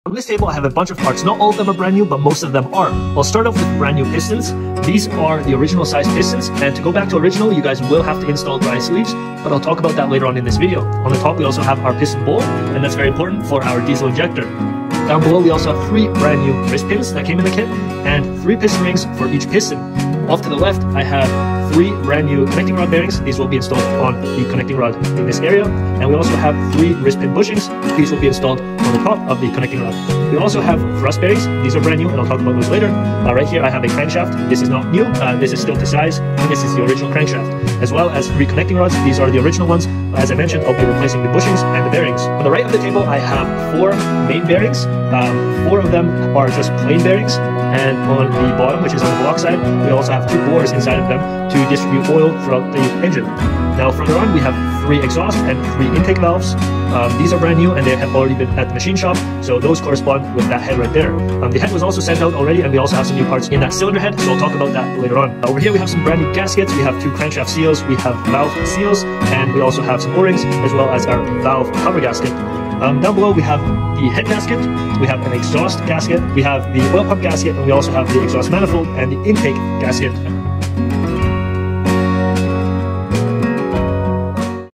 FridayParts X Workshop Rebuild John Deere 855 Yanmar 3TN75RJ Engine Rebuild
A great video on the introduction of engine overhaul components for the Yanmar engine 3TN75RJ.